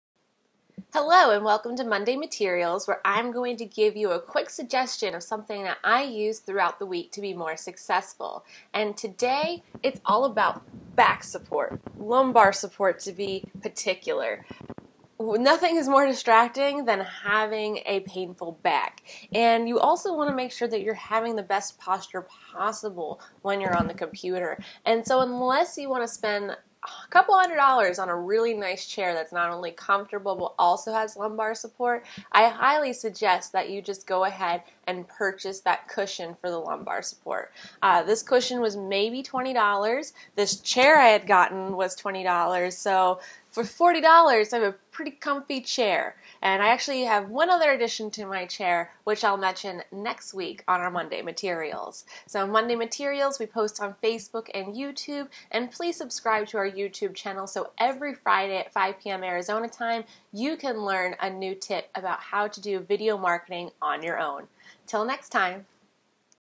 MondayMaterials How To Feel More Comfortable In Your Chair For About $20
MondayMaterials is about how to feel more comfortable in your chair for about $20 and that's to get a cushion for your lumbar support.

There are a lot of different ones out there however this one is great because you can easily strap it onto your chair. I've had it for almost a year now and it hasn't deflated.